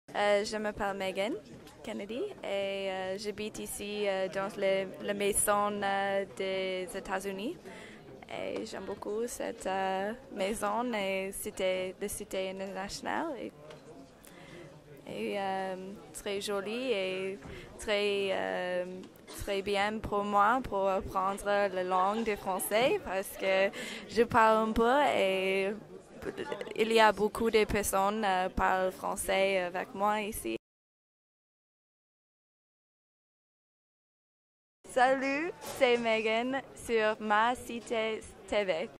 MaCitéTV - Megan K.
Megan - Fondation des Etats-Unis - 12 oct. 2010 - Cité internationale universitaire de Paris